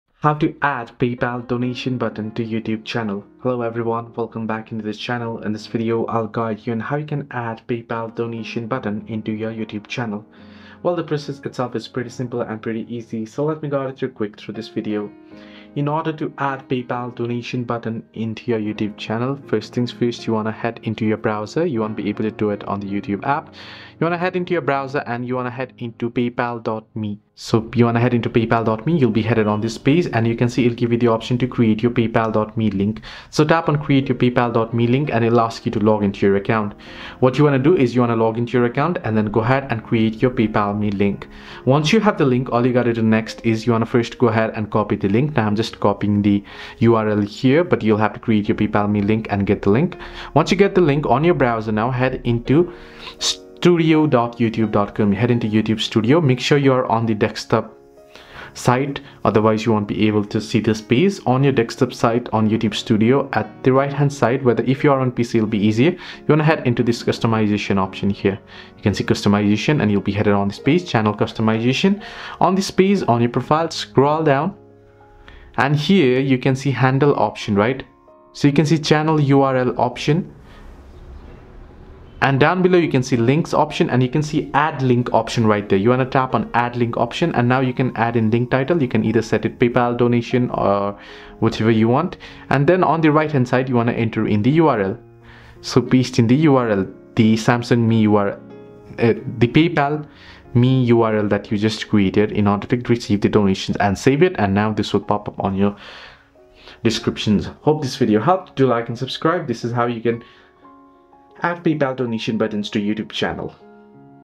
How to Add PayPal Donation Button to YouTube Channel! (Full Guide)
Adding a PayPal donation button to your YouTube channel allows viewers to support you directly. While YouTube does not offer a built-in donation button for PayPal, you can still add a PayPal.me link to your channel’s About section and video descriptions. This is an easy and effective way to receive donations from your audience.

To set up your PayPal donation link, first open a web browser and go to [PayPal.me] Log in to your PayPal account and create a custom PayPal.me link. This link will be used to accept donations. Once created, copy the PayPal.me URL.

Next, open [YouTube Studio] from a desktop browser, as this feature is not available in the mobile app. In YouTube Studio, go to the "Customization" tab and select the "Basic info" section. Scroll down to the "Links" area, then click "Add Link." Here, you can enter a title such as "Donate via PayPal" and paste your PayPal.me link in the URL field. Click "Publish" to save the changes.

Now, when visitors go to your YouTube channel, they will see the PayPal donation link in the About section. Additionally, you can include the same PayPal.me link in your video descriptions to encourage viewers to contribute.

Timestamps:
0:00 Introduction
0:18 Creating a PayPal.me donation link
0:42 Copying the PayPal.me URL
0:53 Adding the PayPal link to YouTube Studio
1:35 Saving and publishing the donation link
1:46 Conclusion